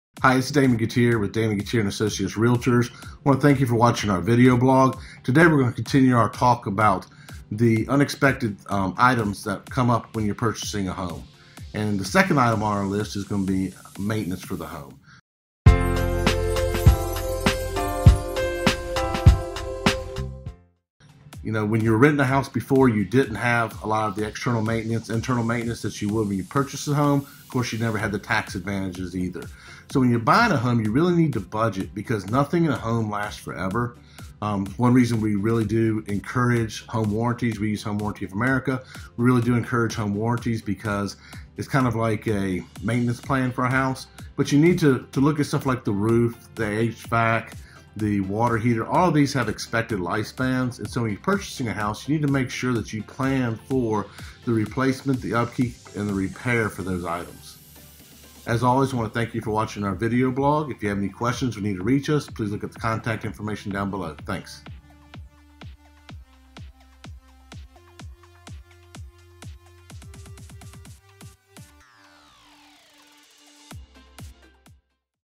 Roanoke Real Estate Agent: Unexpected Home Purchasing Costs, Part II: Maintenance Costs
Today we’re continuing our discussion about unexpected items that come up during a home purchase. This time, we’ll focus on maintenance for the home. When you rent a house, you don’t have the same external and internal maintenance duties you have when you buy a house. That’s why it’s important that you budget accordingly once you decide to become a homeowner. To learn more about preparing for maintenance costs during your home purchase, watch my latest video.

Buying a Central or Southwest Virginia Area home?

Selling your Central or Southwest Virginia Area home?

Damon Gettier
Damon Gettier & Associates
1611 E Main St